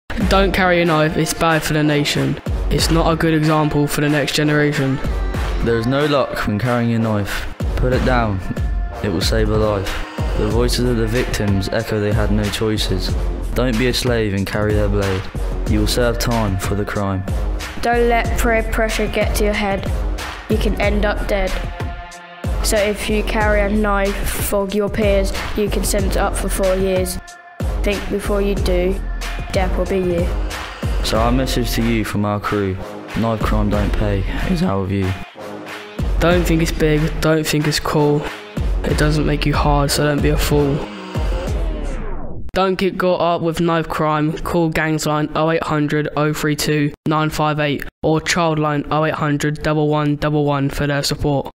Havant Academy
This is one of the fantastic finalists in the Hampshire schools Knife Crime Media Challenge, in association with Hampshire Constabulary and the Office of the Police and Crime Commissioner.  Watch as many as you can and please vote for your favourite